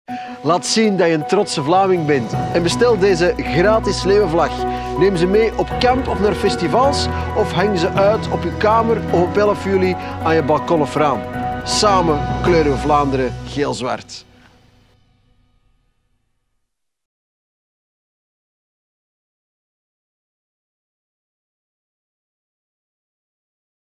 Kleur Vlaanderen Vlaams
Binnen enkele weken vieren we weer 11 juli, onze Vlaamse feestdag. én hang hem uit je raam op 11 juli. Samen kleuren we Vlaanderen Vlaams.